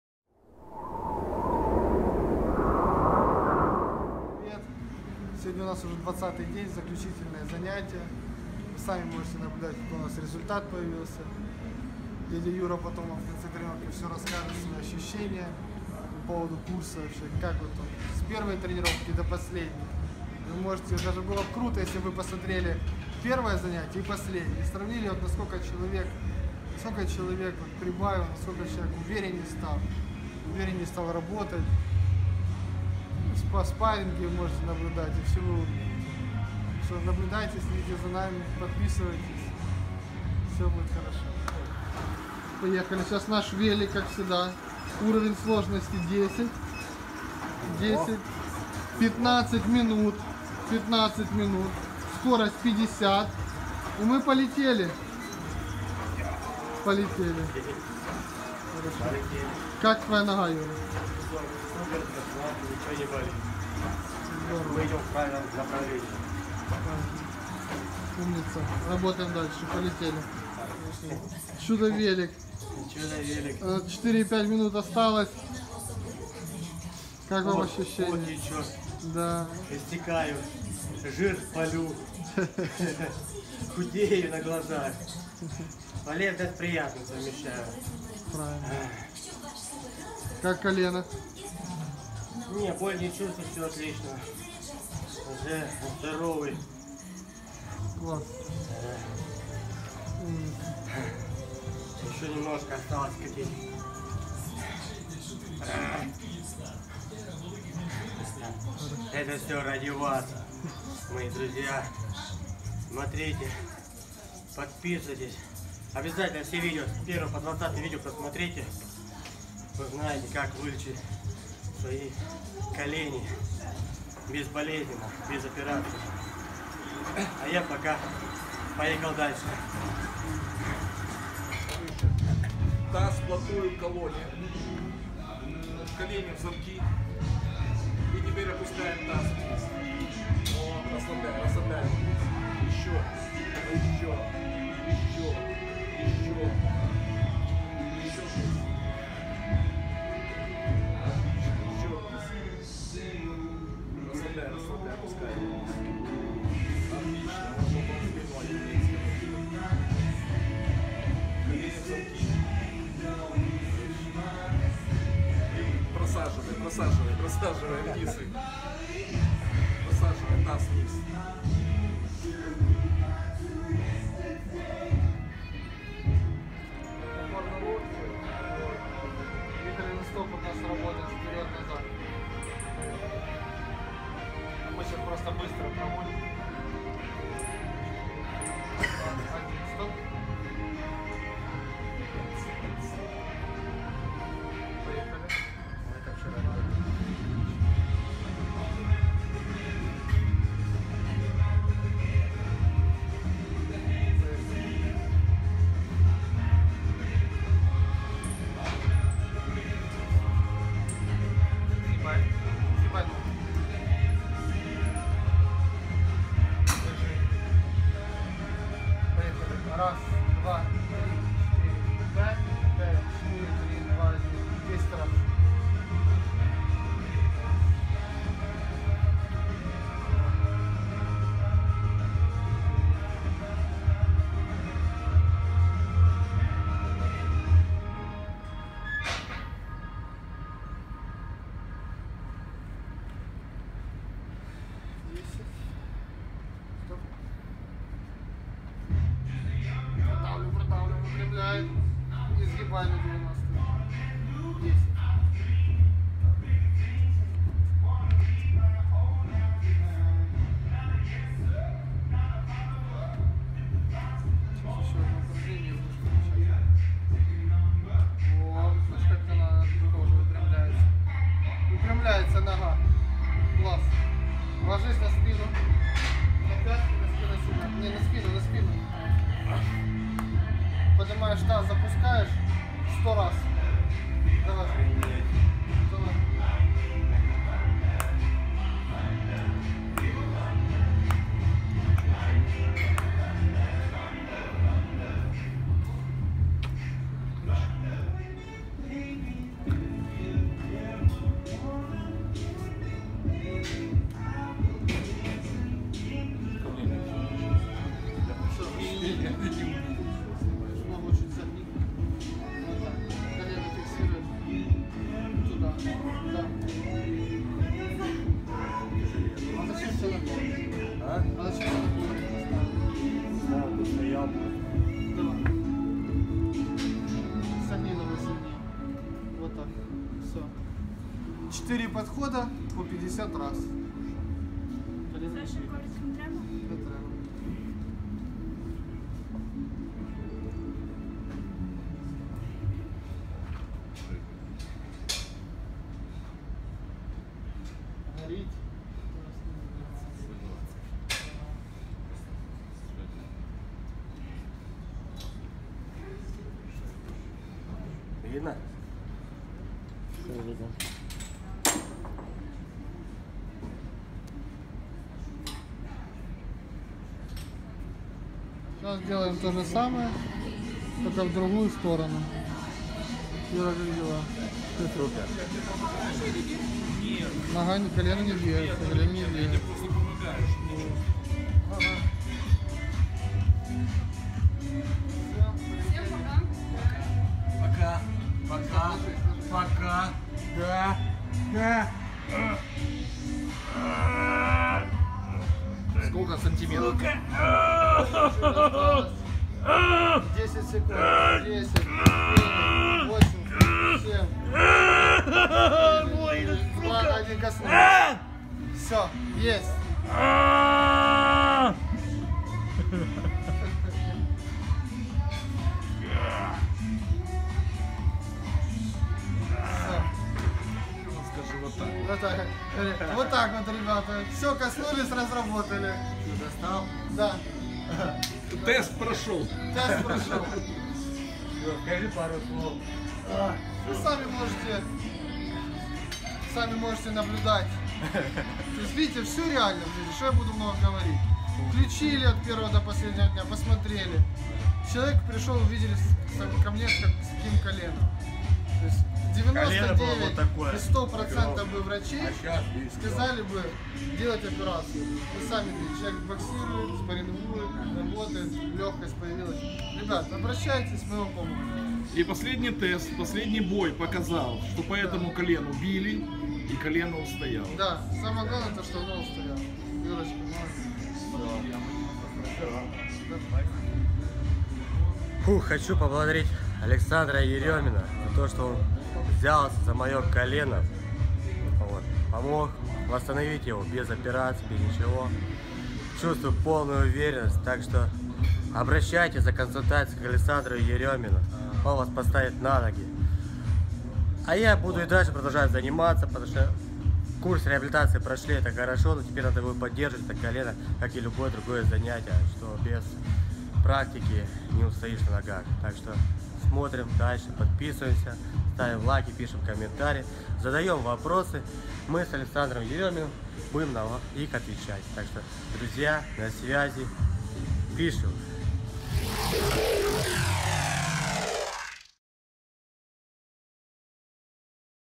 Реабилитация колена . День №20
20 дневный курс. День №20
Полезные рекомендации от Александра Еремина по реабилитации травмированного коленного сустава..